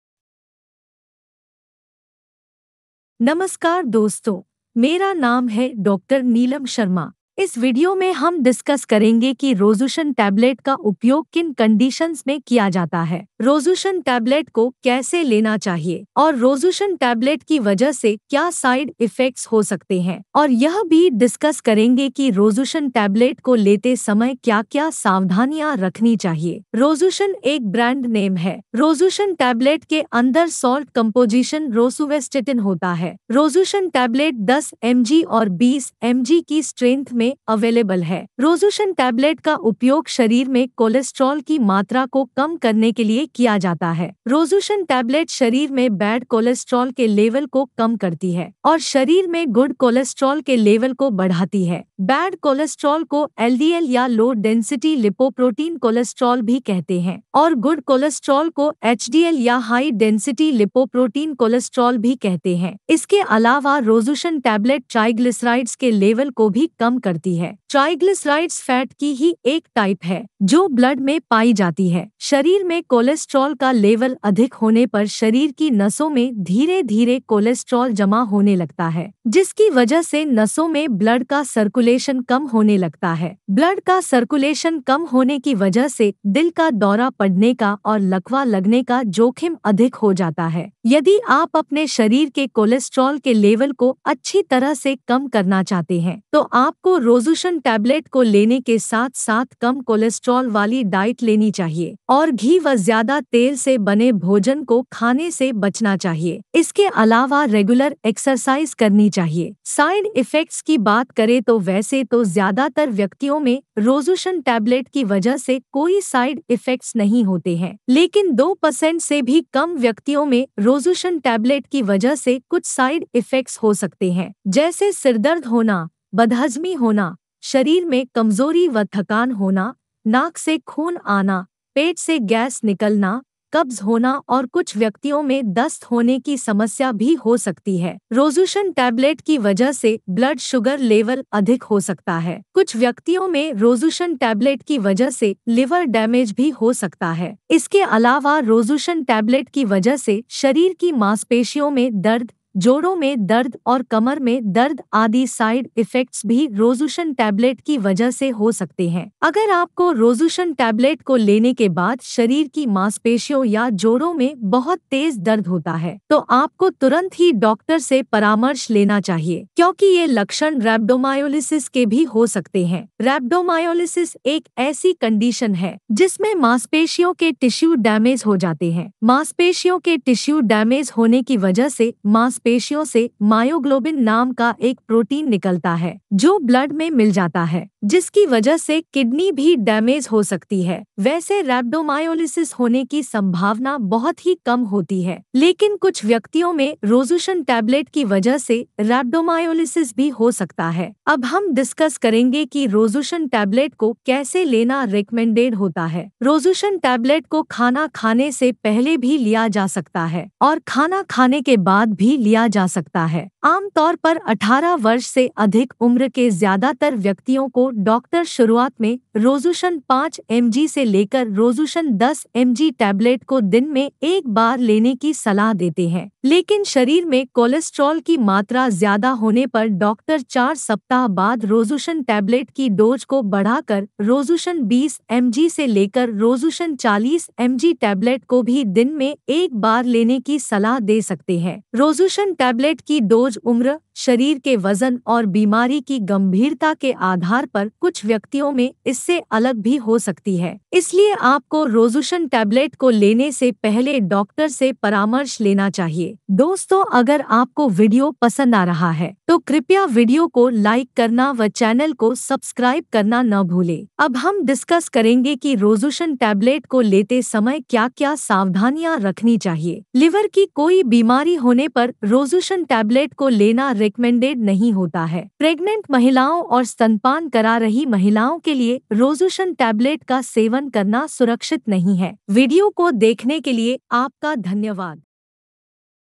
Rozution 10 mg, 20 mg Tablet Uses & Side Effects in Hindi
in this video, we will discuss the uses and benefits of rozution tablet. How to use rozution tablet. We will also discuss what are the side effects of rozution tablet. Watch the complete video for detailed review of rozution tablet in hindi.